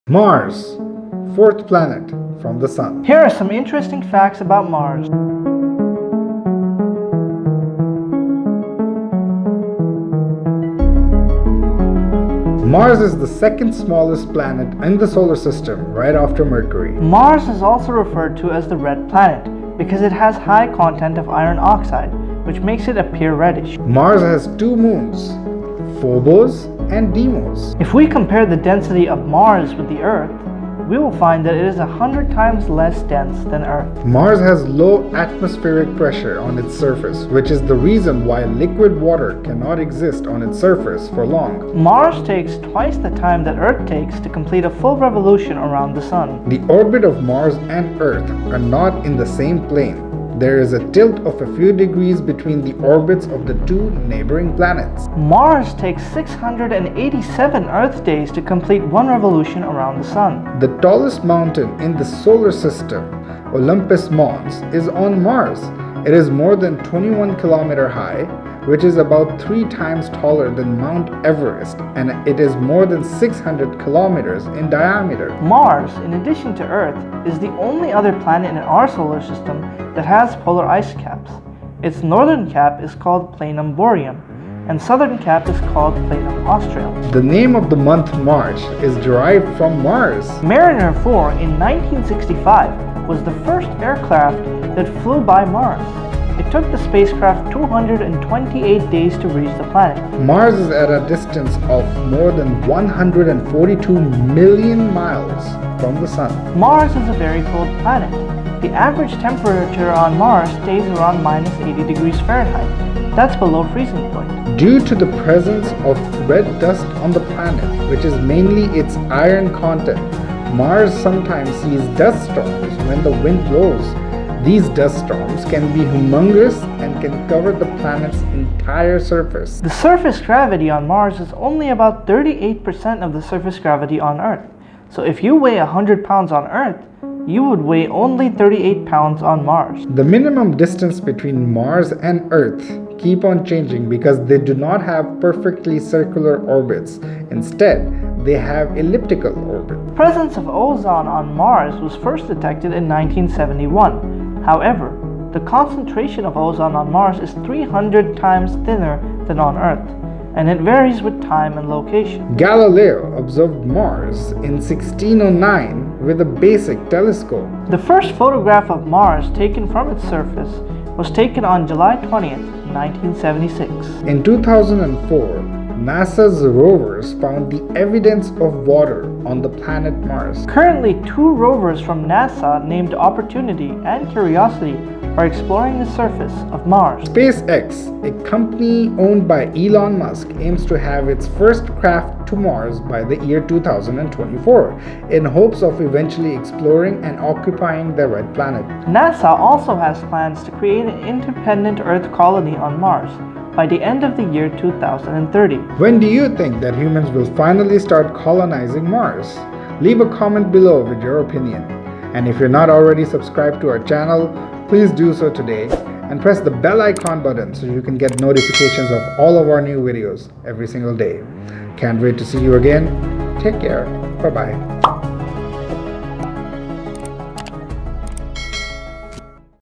Interesting Facts about Mars
We are back with another interesting facts video, this time on the planet Mars. How much do you really know about Mars? Here are some interesting facts about Mars.

All images, videos, music, and so on used in the video are being used under free use.